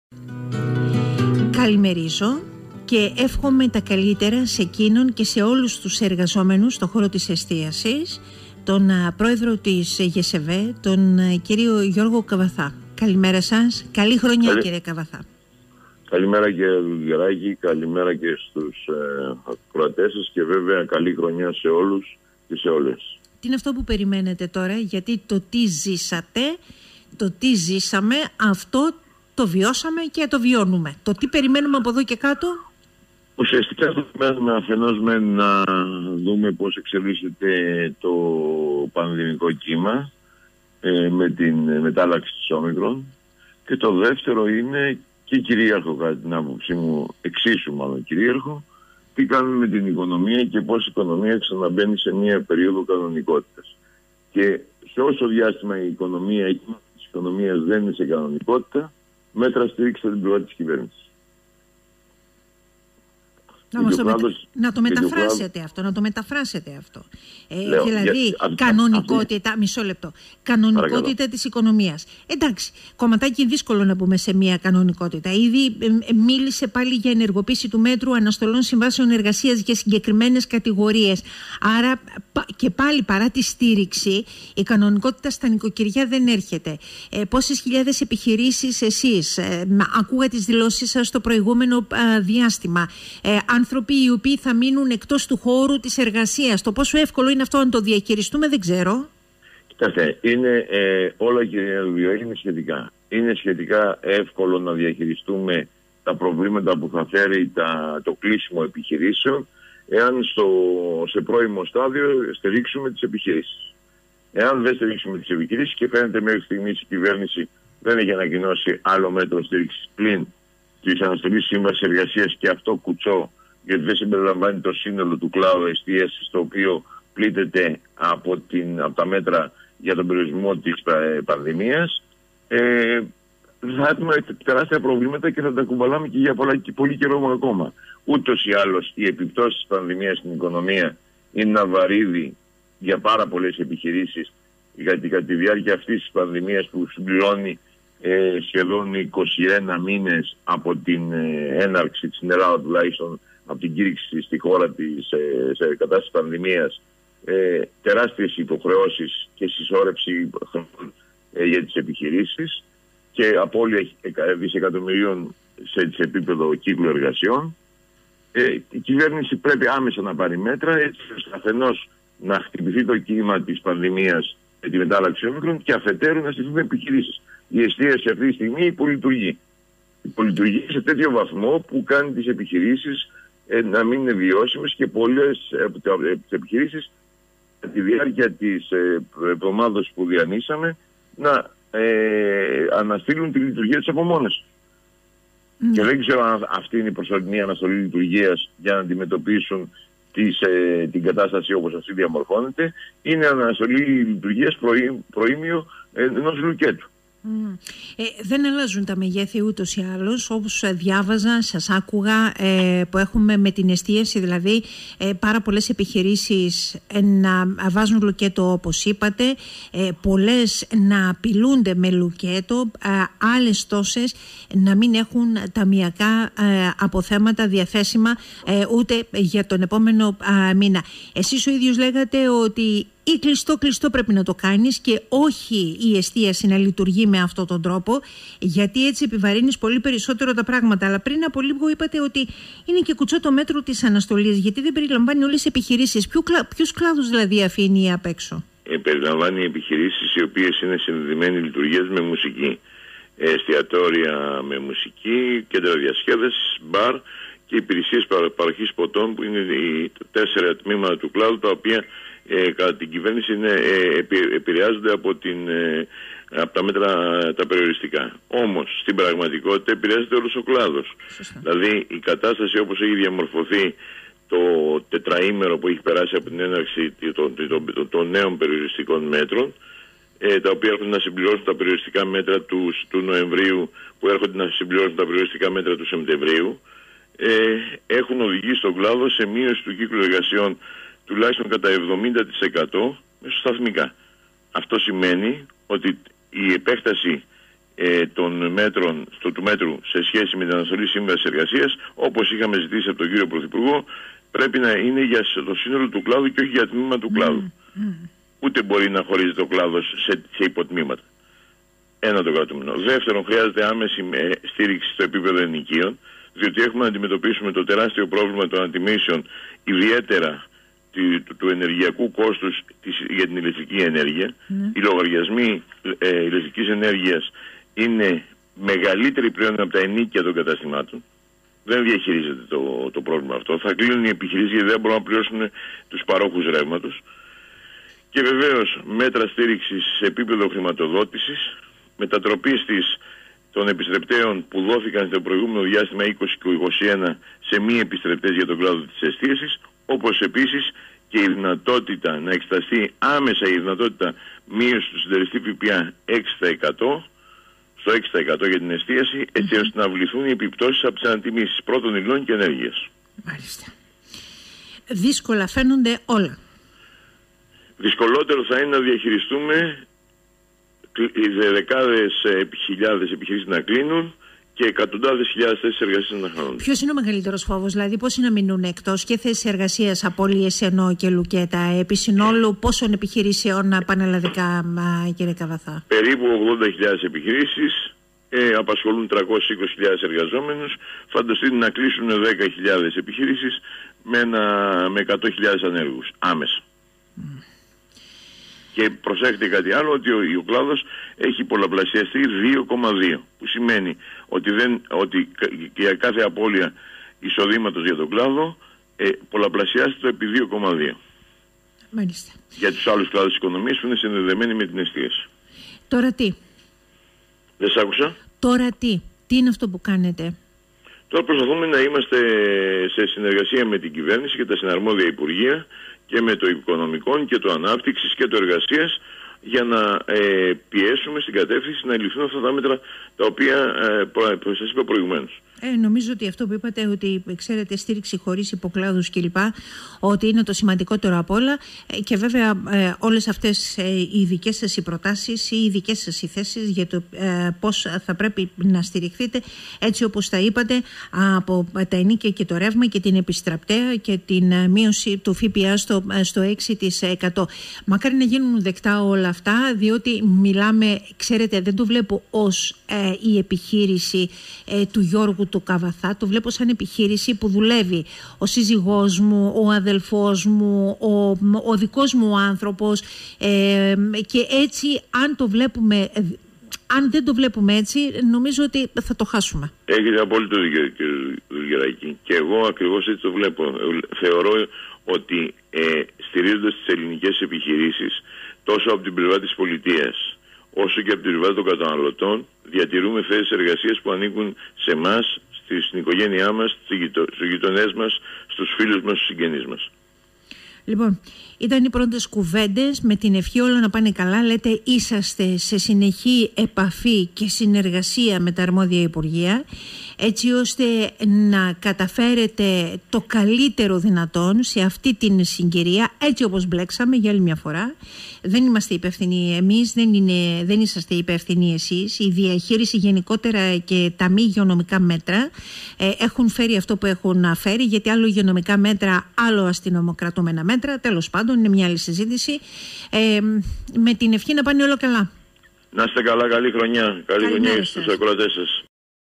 Γ. Καβαθάς: Η εστίαση χρειάζεται στήριξη χωρίς υποκλάδους
Κουτσά τα μέτρα που πήρε η Κυβέρνηση

Ενεργοποίηση του μέτρου αναστολών συμβάσεων εργασίας με αποζημίωση ειδικού σκοπού, τηλεργασία και επέκταση του προγράμματος «ΣΥΝ-ΕΡΓΑΣΙΑ» είναι μεταξύ των μέτρων που ανακοινώθηκαν από τα Υπουργεία Οικονομικών και Εργασίας και Κοινωνικών Υποθέσεων για τη στήριξη της εστίασης.
Στήριξη χωρίς υποκλάδους ζητά ο Πρόεδρος της ΓΣΕΒΕΕ Γιώργος Καβαθάς, μιλώντας στον 98.4, καθώς το μέτρο της αναστολής είναι κουτσό, όπως λέει ενδεικτικά.
Αναφέρεται στα λουκέτα που μπήκαν κι αυτά που έρχονται, με τις 80.000 επιχειρήσεις πανελλαδικά που απασχολούν 320.000 εργαζόμενους να ζητούν: «Τη στήριξη του συνόλου των επιχειρήσεων, σε ενοίκια, ρεύμα μείωση ΦΠΑ στο 6%, επιστρεπτέα».
Προσθέτει ότι μία στις τρεις επιχειρήσεις έχει μηδενικά ταμειακά διαθέσιμα και τέσσερις στις δέκα έχουν αβεβαιότητα για το μέλλον τους και πως σε συνεργασία με τα συναρμόδια υπουργεία προσπαθούν να βρουν τη βέλτιστη λύση.